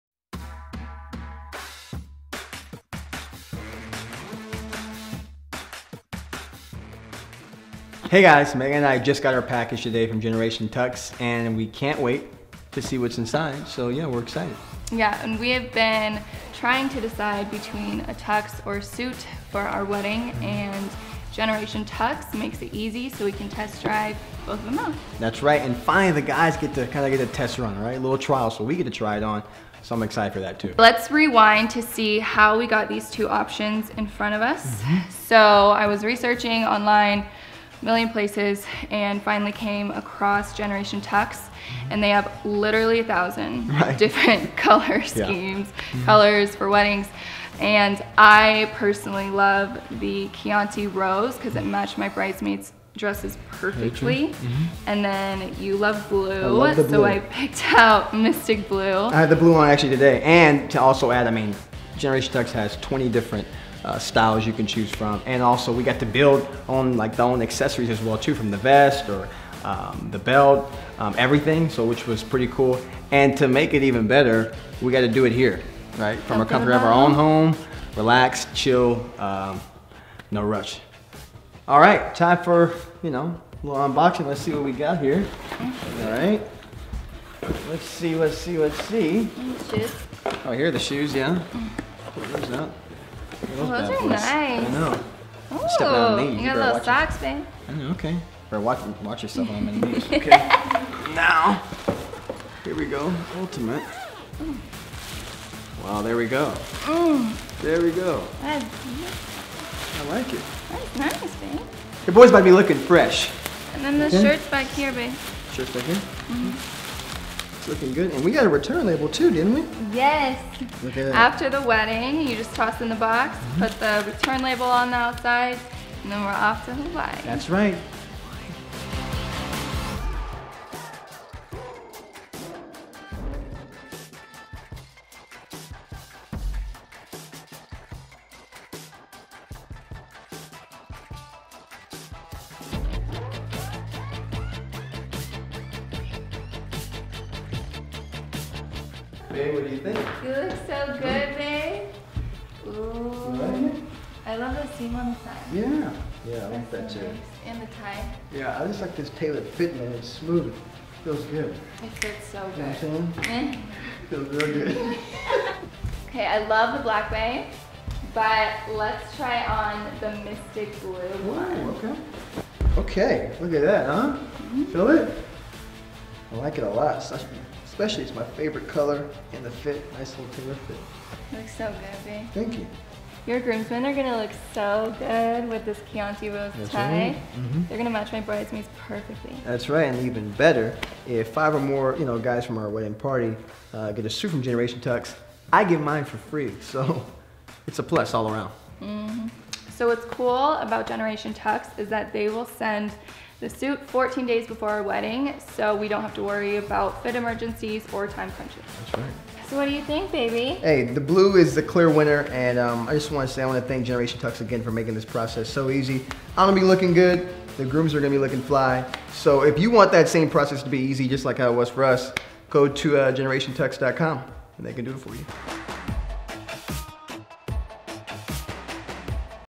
Generation Tux Unboxing - Wedding Day Look For the Groom
We just received our shipment from Generation Tux - we're going to try on a suit and tux and then pick which is best for our wedding day!